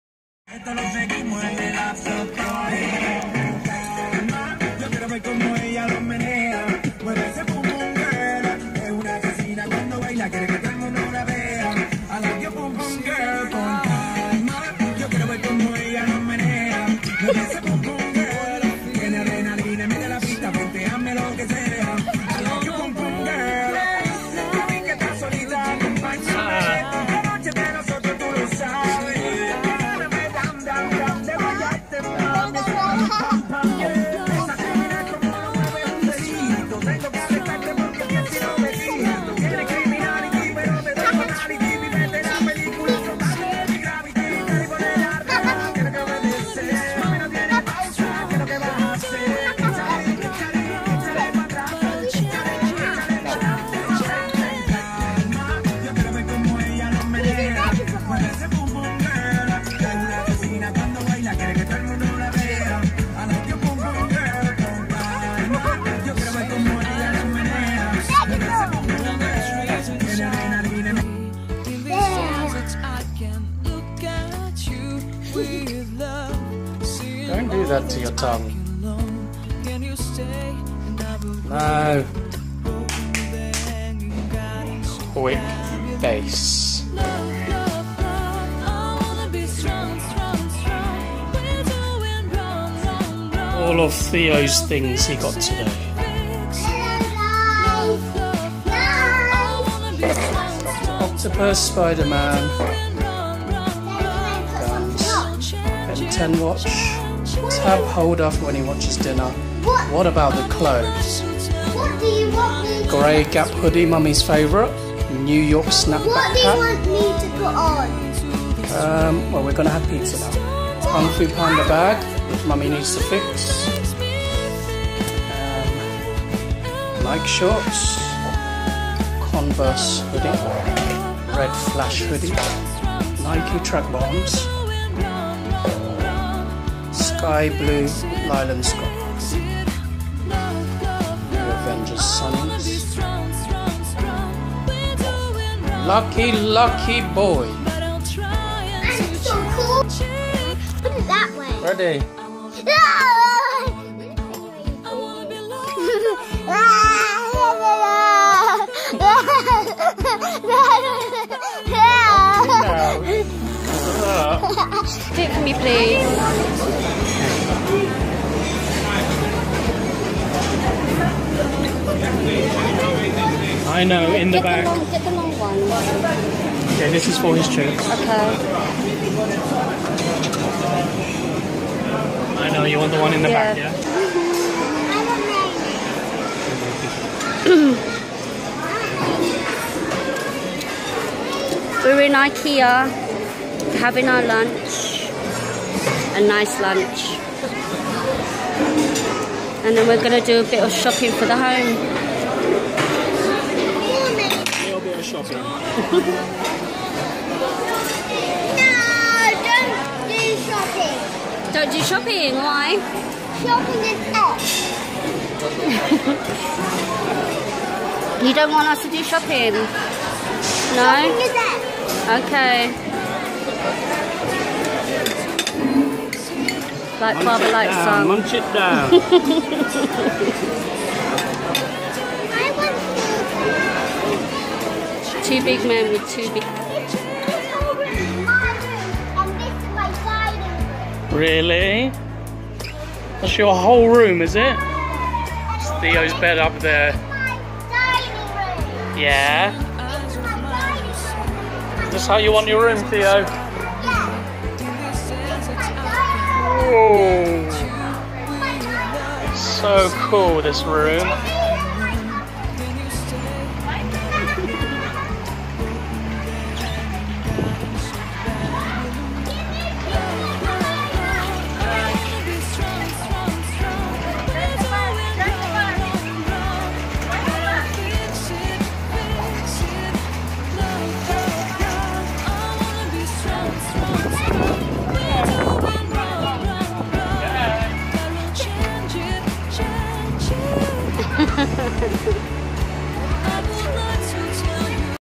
Playing with Kids Toys and going on the Merry go round
Hi guys!

Take a look at some of the fun I got upto in the weekend!

Theo, 😊 🙌